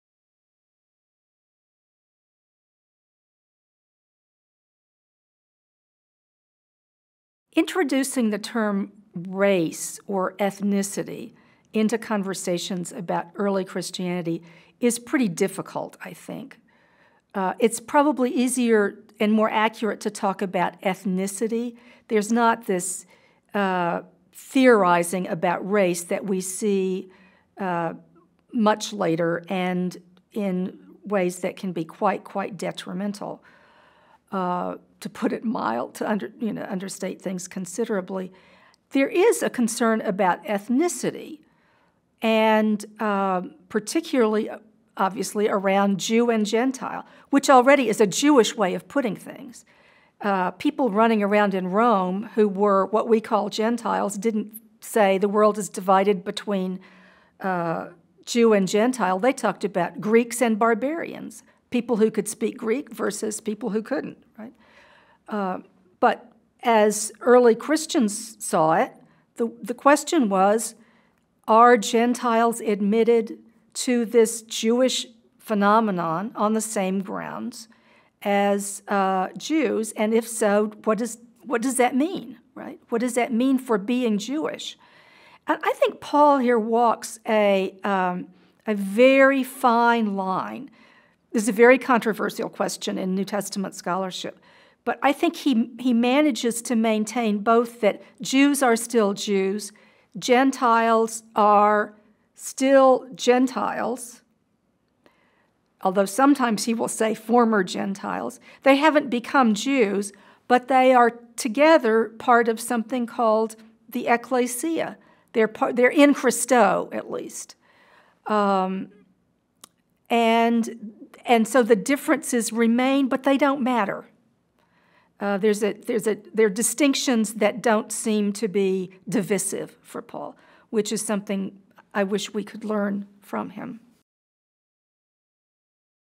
On ethnicity
Beverly Gaventa talks about division and unity among the first Christians.